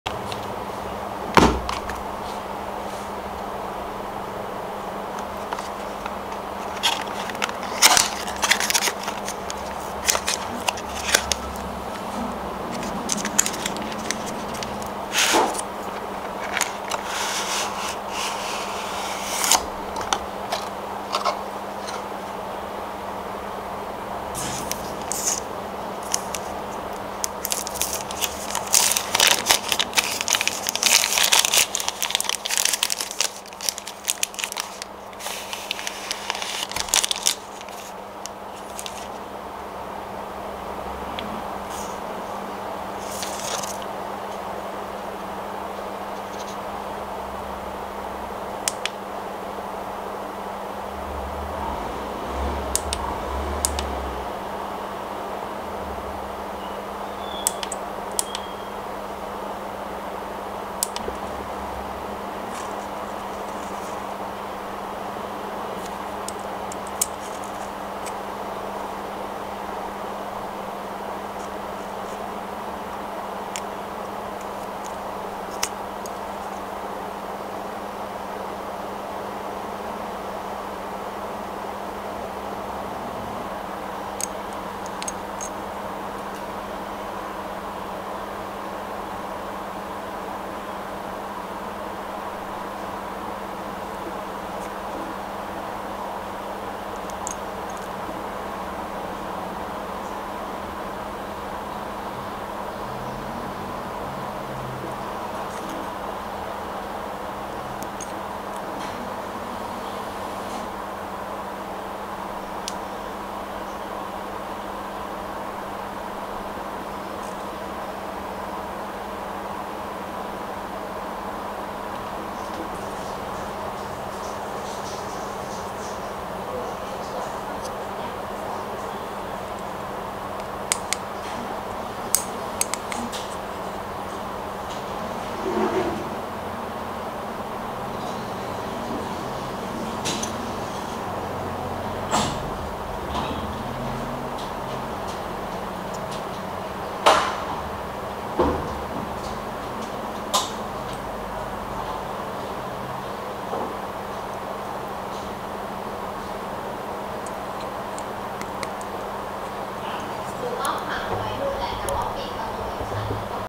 [ ASMR ] Dspiae UV Touch Light
DSPIAE UV Touch Light ไฟฉาย UV แถมยังเป็น Power bank ในตัวอี๊กกก
ใครสนใจก็ไปตำกันได้เลยครับตามพิกัดด้านล่างนี้ค้าบ